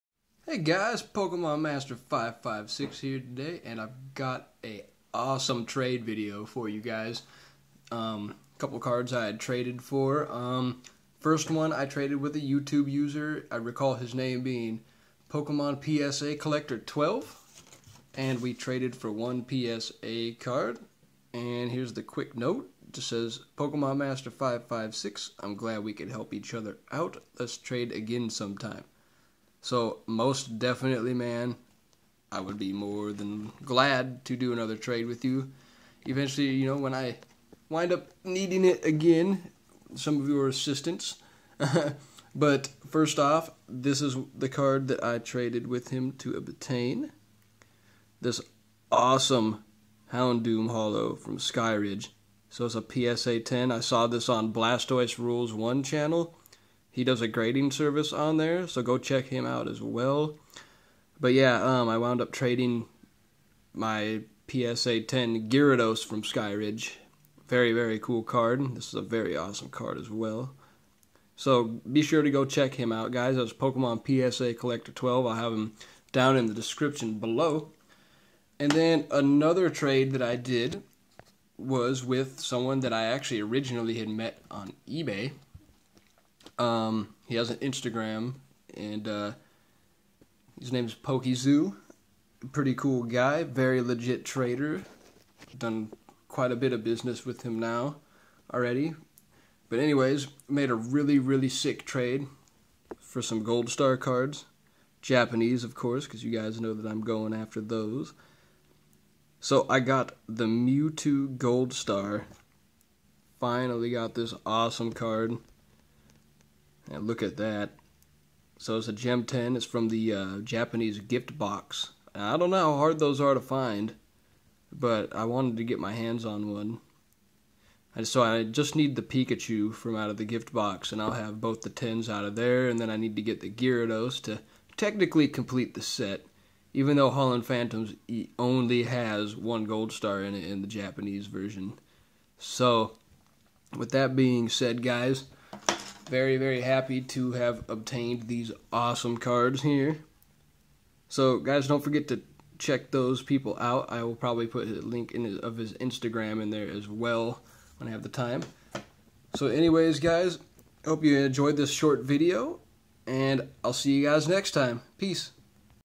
Pokemon TCBM Pokemonpsacollector12 Psa 10s
Hey everybody! So I made a couple of really sweet trades with people and picked up some super epic cards. Go check out this dudes channel. I hope you all enjoy this quick video.